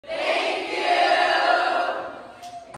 A warm Giving Tuesday thank you from Notre Dame's Class of 2023!
To our wonderful community of Giving Tuesday supporters, the class of 2023 offers a hearty THANK YOU!